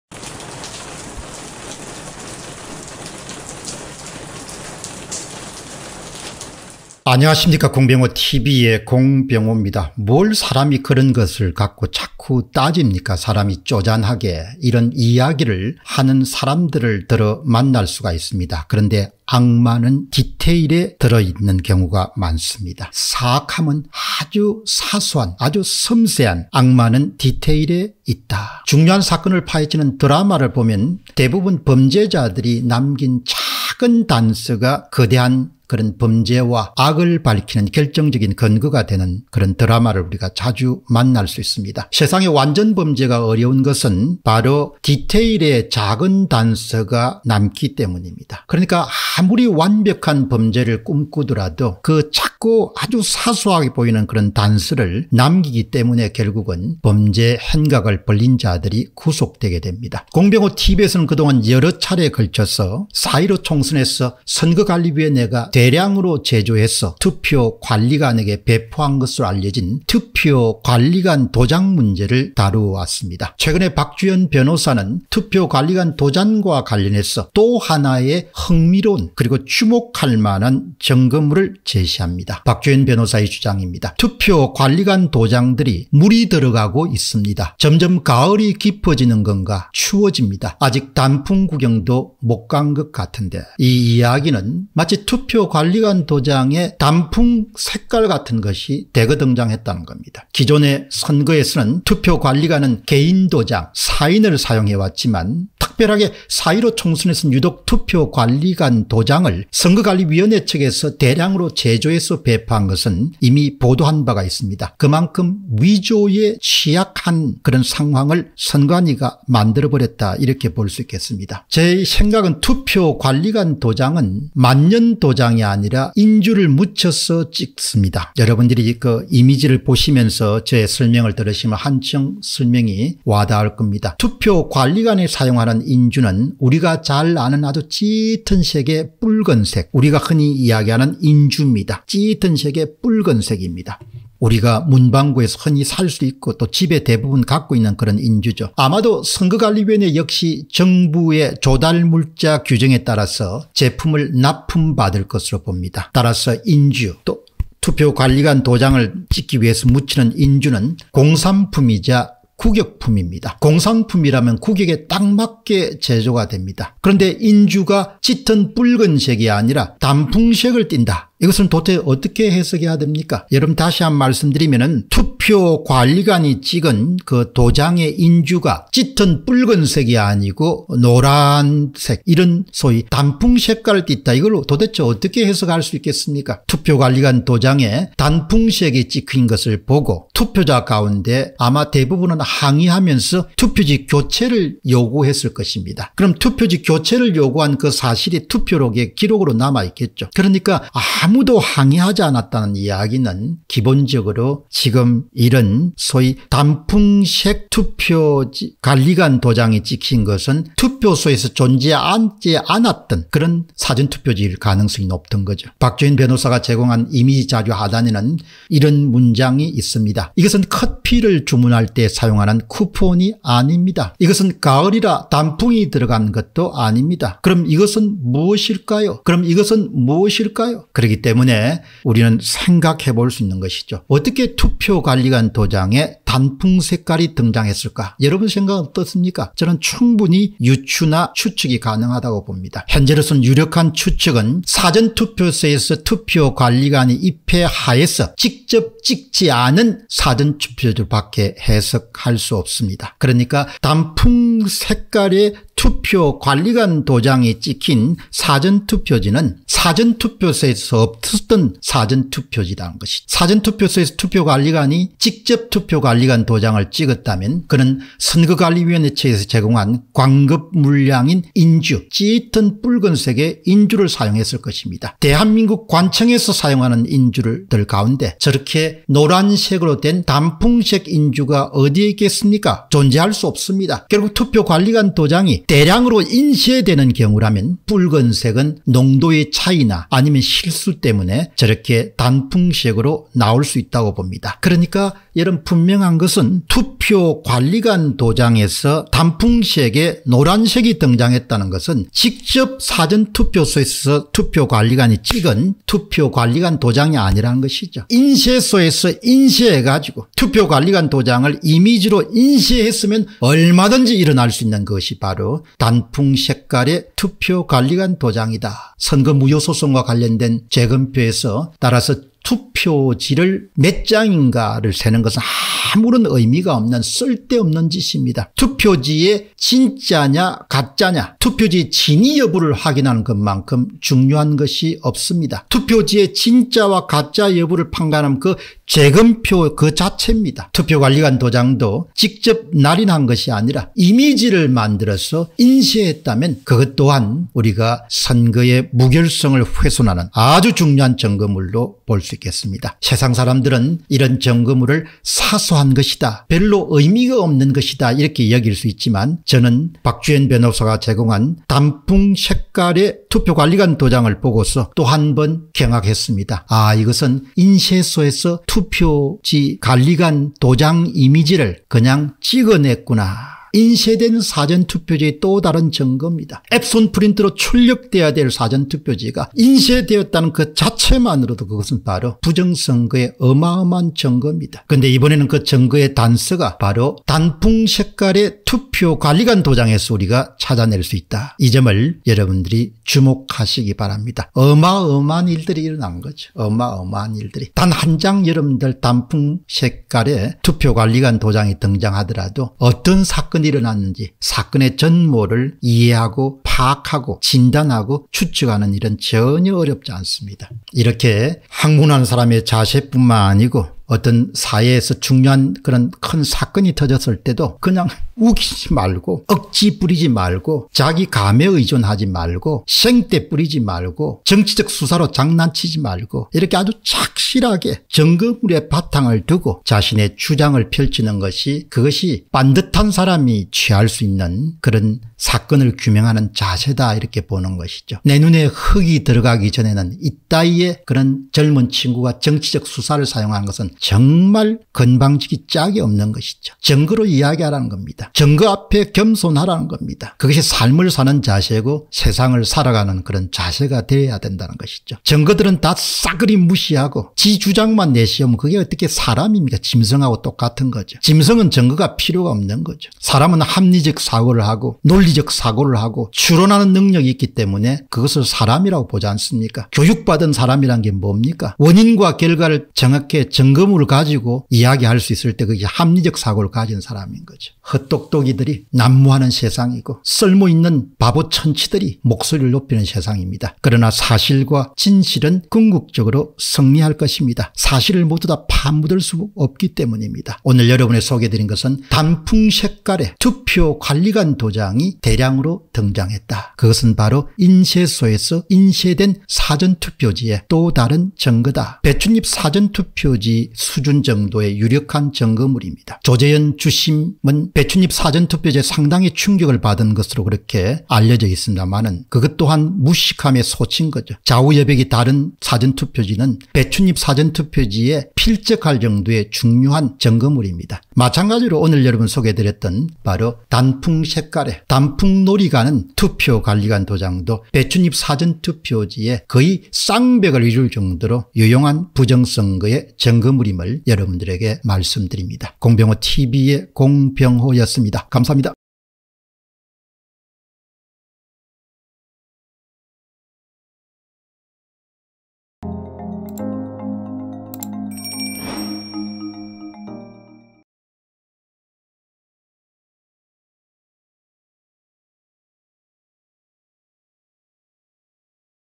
투표관리관 도장 / 가을단풍색 도장 / 어째 도장 인주가 가을인가 / 인쇄된 경우에 발생할 수 있는 특별한 현상 [공병호TV]
Bitcoin : 3MTW849AkP2ftLeSK2teZHMmzLu7Z83s9U